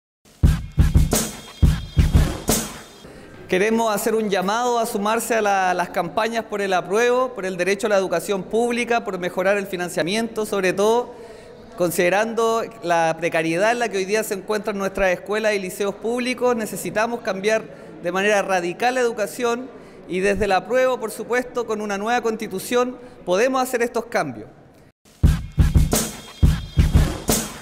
[VOCES POR EL APRUEBO] «NECESITAMOS CAMBIAR LA EDUCACIÓN Y CON UNA NUEVA CONSTITUCIÓN PODEMOS»
[VOCES POR EL APRUEBO] «NECESITAMOS CAMBIAR LA EDUCACIÓN Y CON UNA NUEVA CONSTITUCIÓN PODEMOS HACER ESTOS CAMBIOS»

Leandro Silva, Tesorero del Regional Valparaíso del Colegio de Profesoras y Profesores, plantea la urgente necesidad de cambios en la Educación y como el Apruebo y la Nueva Constitución posibilitan esa necesaria transformación.